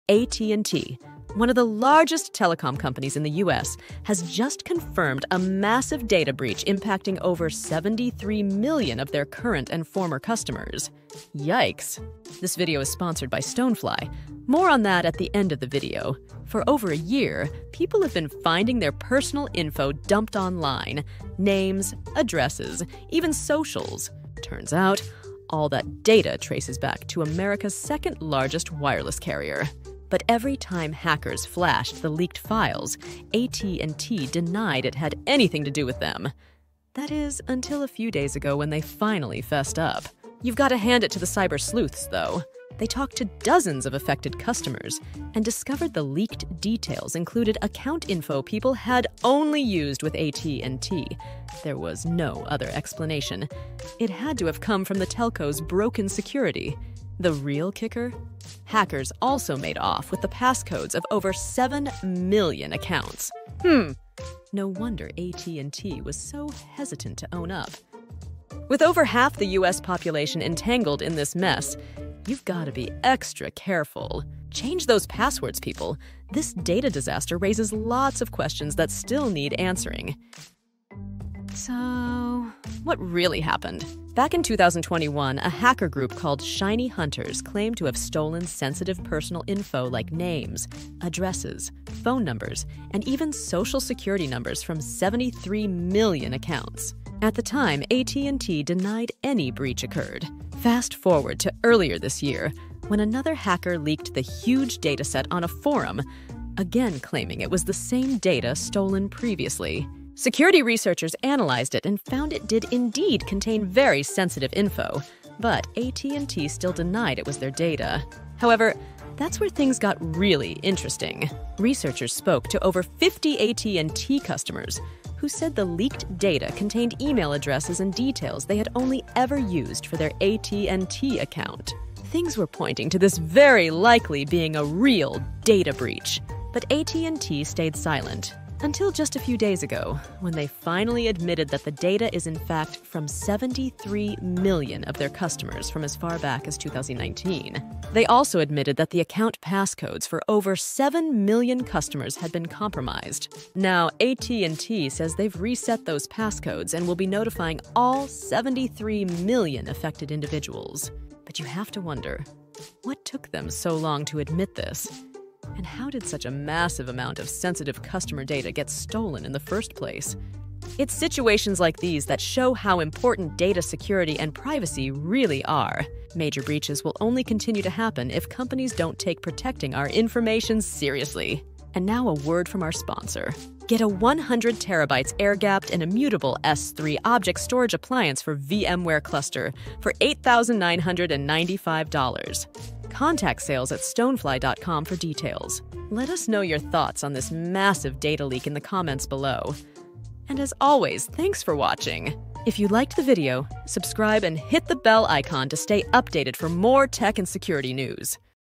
AT&T Admits: Over 73 MILLION Accounts Compromised!
AT&T, one of the largest telecom companies in the US, has just confirmed a massive data breach impacting over 73 million of their current and former customers!

For over a year, people have been finding their personal info dumped online - names, addresses, even socials. Turns out, all that data traces back to America's second largest wireless carrier.

But every time hackers flashed the leaked files, AT&T denied it had anything to do with them! That is, until a few days ago, when they finally fessed up.